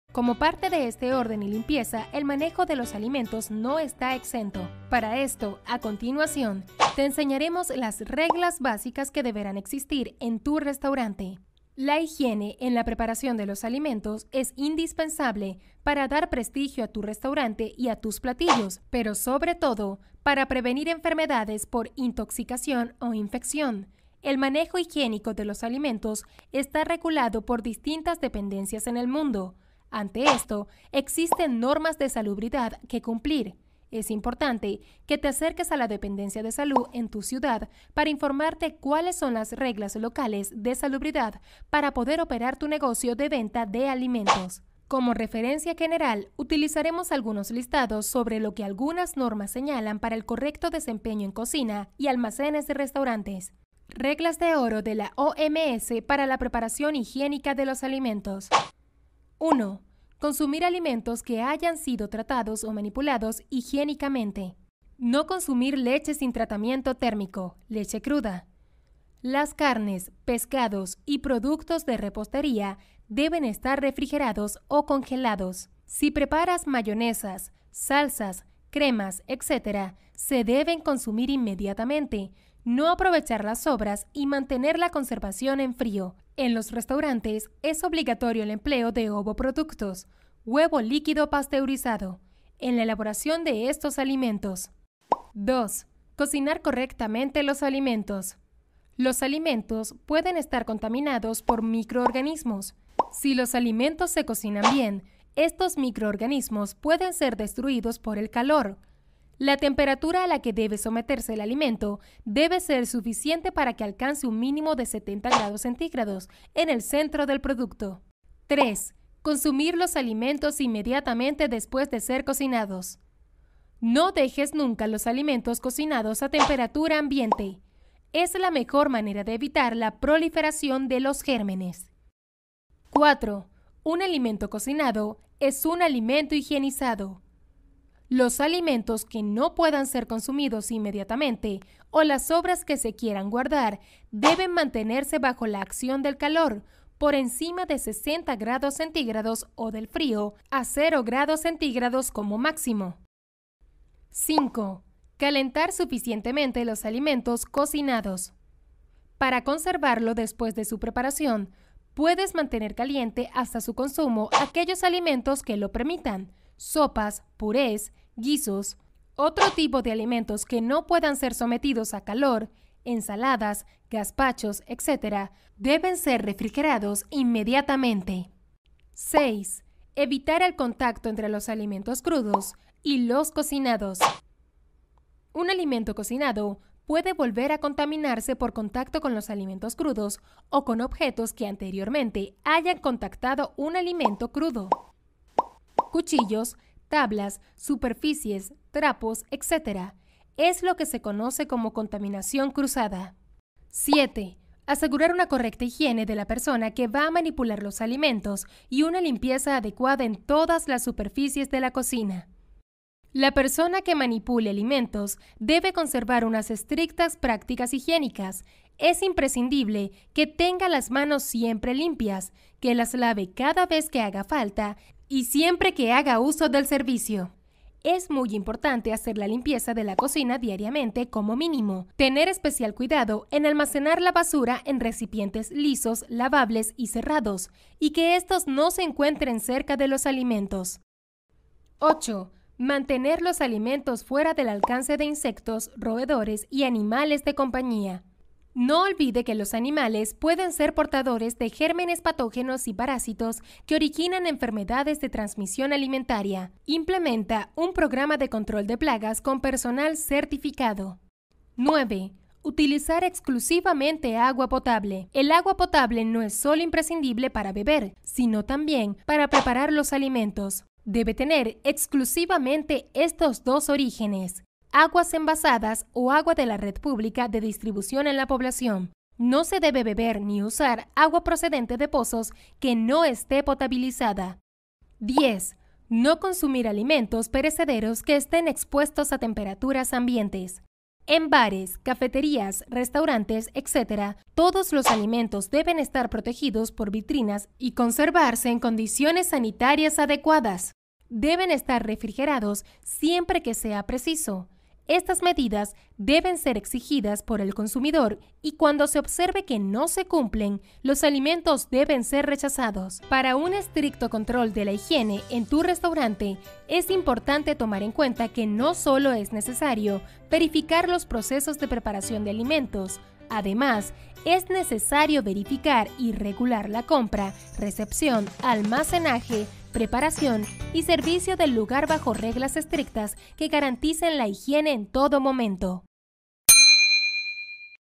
Manejo higiénico de los alimentos en un restaurante
El Manejo higiénico de los alimentos en un restaurante.
Muchos son los factores que contribuyen a los problemas por intoxicación y enfermedades gastrointestinales por consumo de Alimentos en Restaurantes y Negocios de Comida, entre ellos la refrigeración inadecuada de los insumos, la higiene limitada de los cocineros, la cocción insuficiente, la manipulación y conservación de los alimentos en las zonas de peligro o temperaturas de riesgo, la contaminación de alimentos crudos con cocinados (contaminación cruzada), la incorrecta limpieza de los equipos y los utensilios, y la utilización de alimentos provenientes de fuentes inseguras.

Protocolos d el manejo higiénico de los alimentos en tu restaurante o negocio de comida. Distintivo H